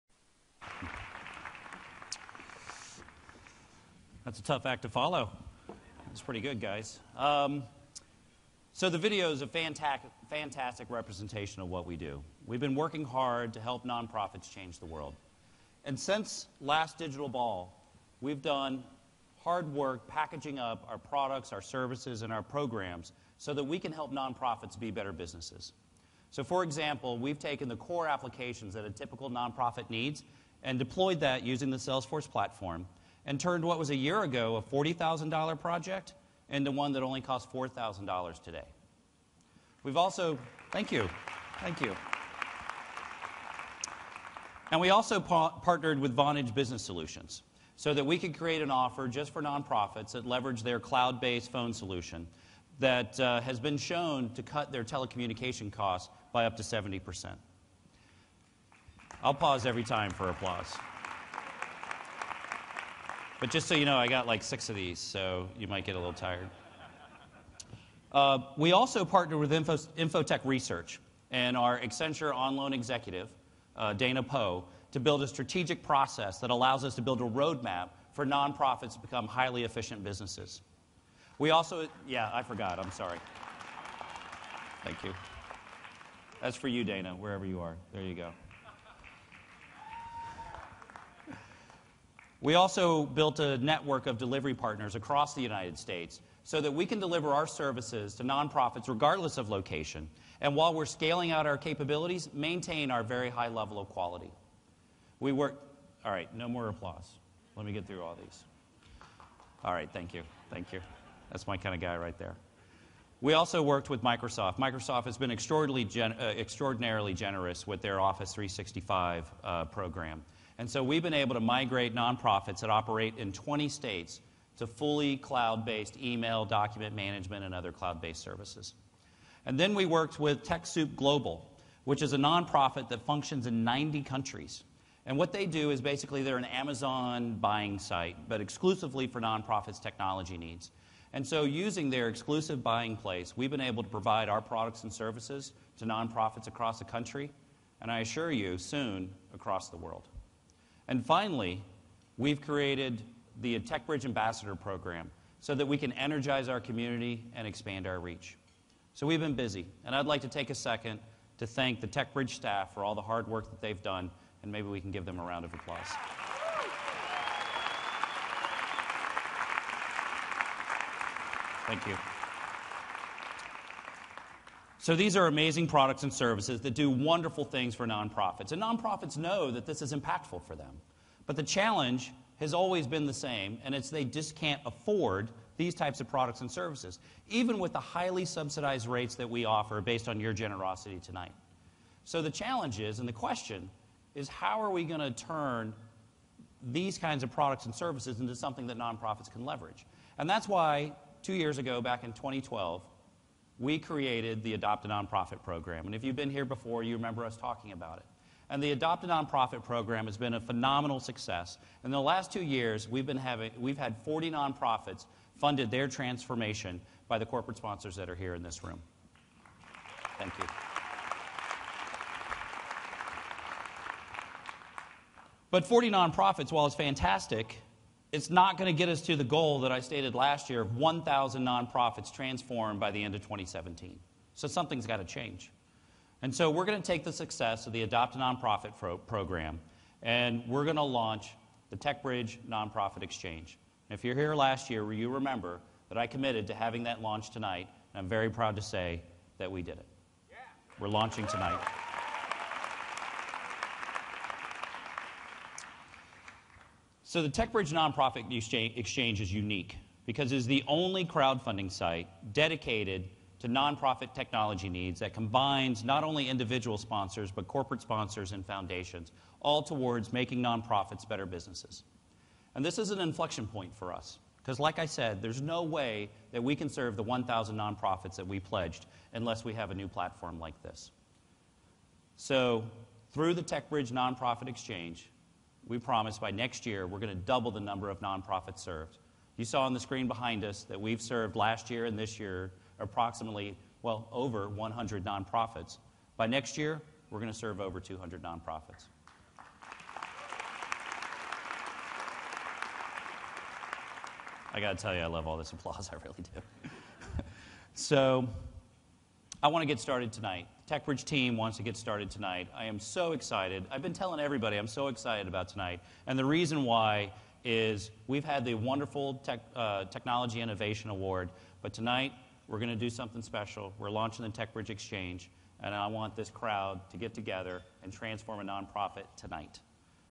2014 Digital Ball - James Franklin, CEO of TechBridge
2014 Digital Ball - James, Franklin, CEO of TechBridge, addresses the crowd.